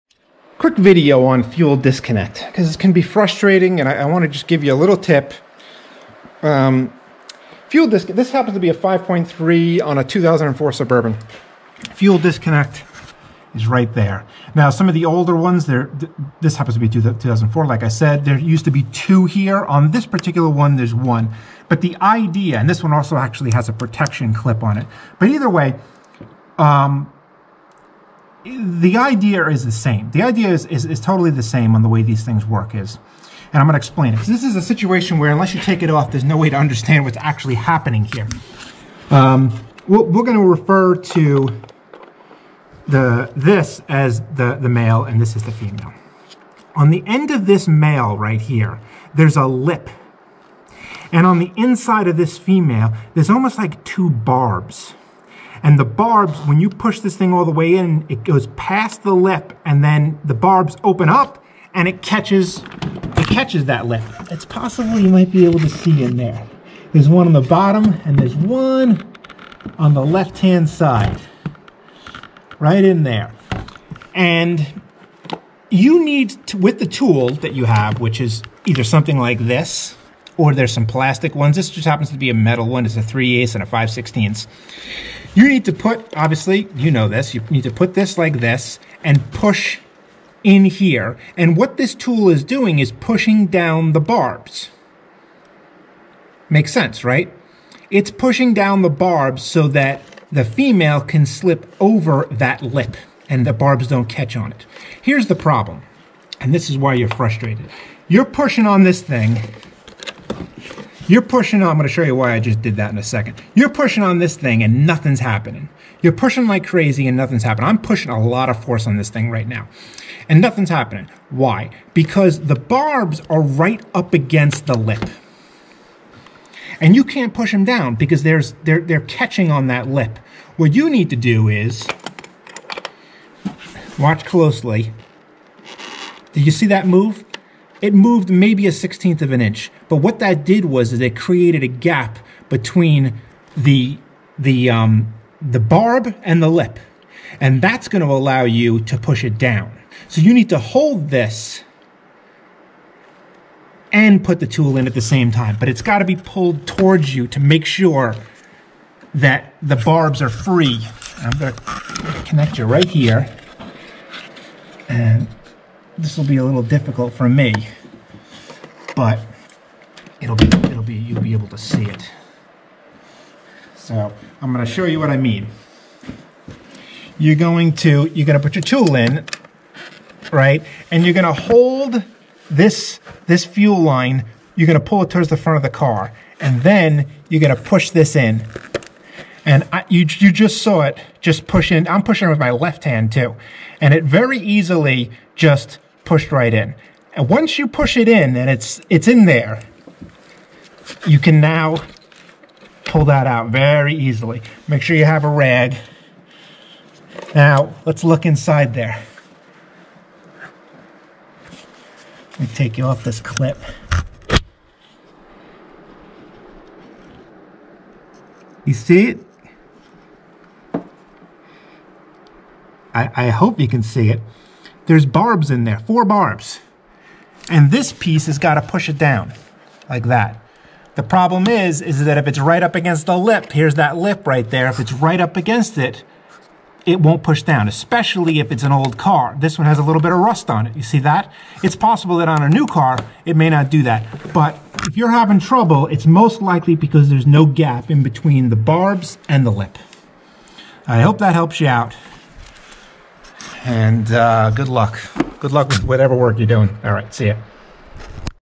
Here's Why Your Fuel Line Disconnect is Stuck - Simple Solution Trick
Fuel Line Disconnect Tool:

Or Here’s a Full Kit for all cars and AC plus transmission

Fuel Line Disconnect can be a real pain. But if you understand how it the connection works, it's actually very easy to get them apart. Hopefully this trick helps you.